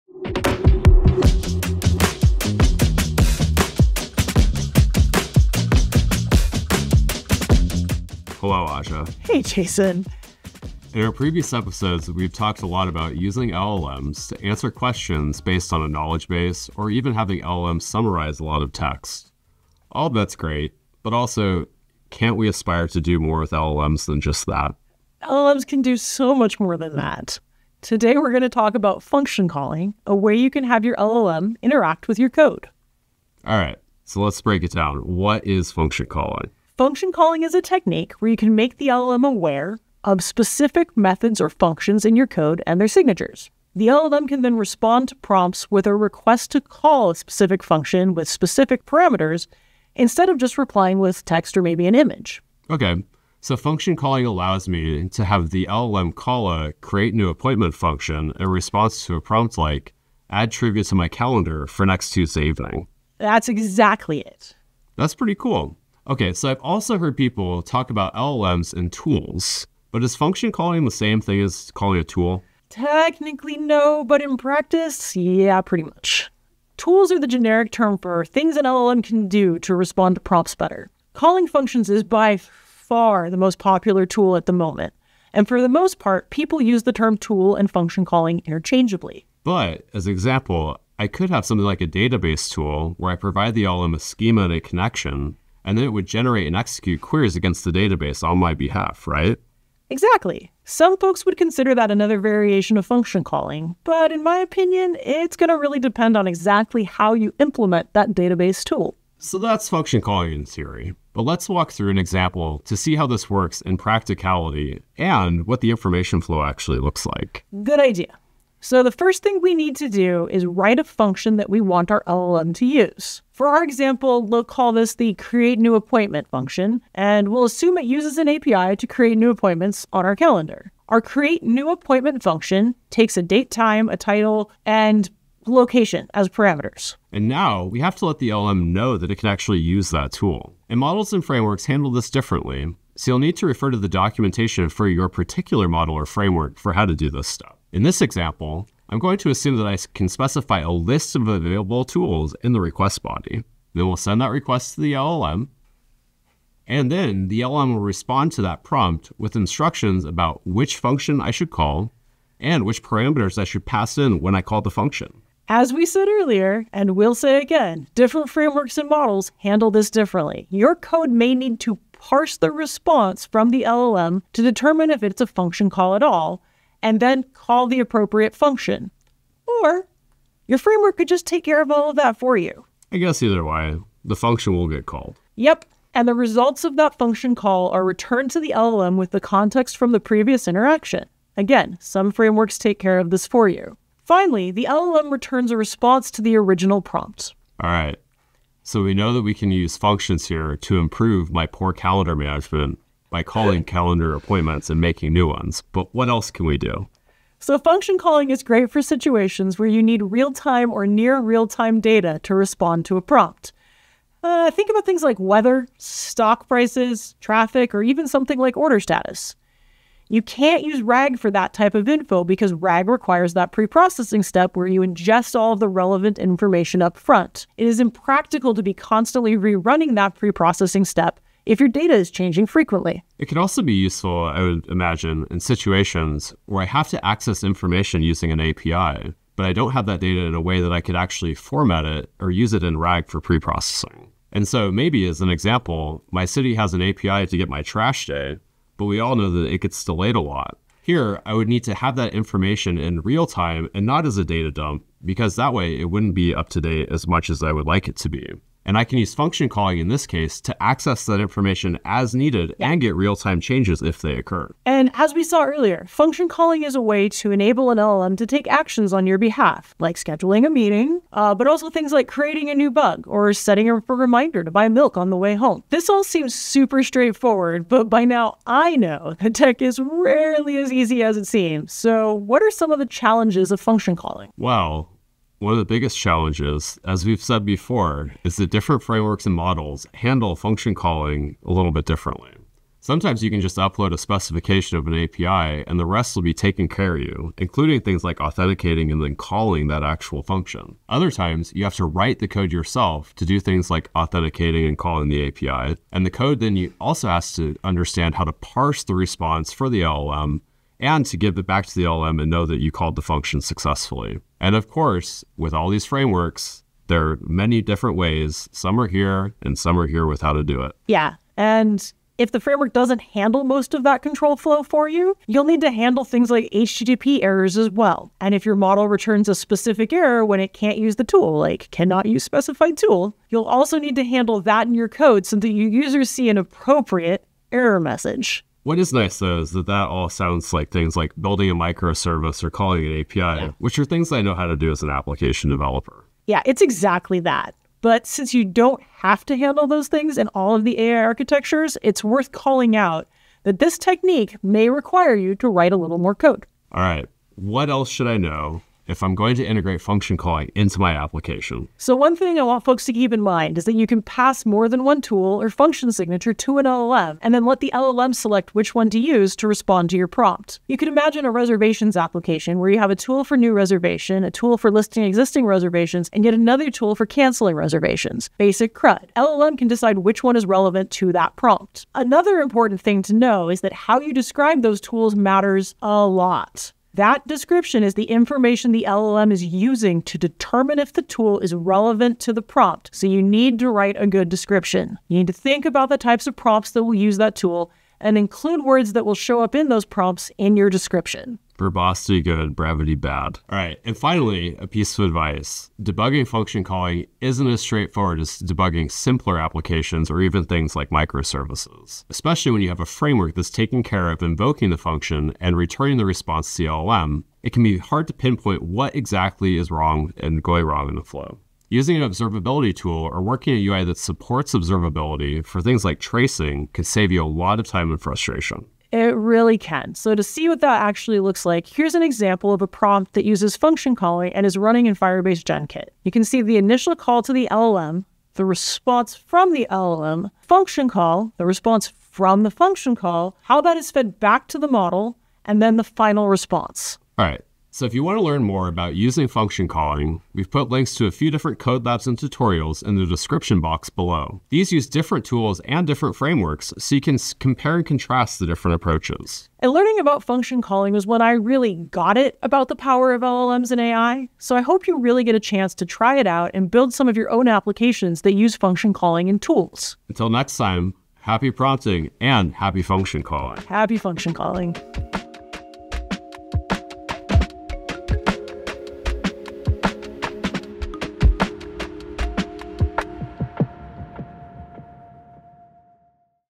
AI + your code: Function Calling
Explore the world of function calling, a powerful technique in AI that allows large language models (LLMs) to interact with code and external APIs. Discover how this technique allows AI systems to perform a broader range of tasks, handle real-time information, and even execute actions in response to user prompts.

Chapters:
0:00 - Intro
0:32 - What is function calling?
1:48 - Function calling information flow
3:17 - Why use function calling?
4:35 - Challenges with function calling
5:55 - Extra tips for using function calling
7:27 - Get started today

Speakers: Aja Hammerly, Jason Davenport
Products Mentioned: Cloud - AI and Machine Learning - Function Calling, Gemini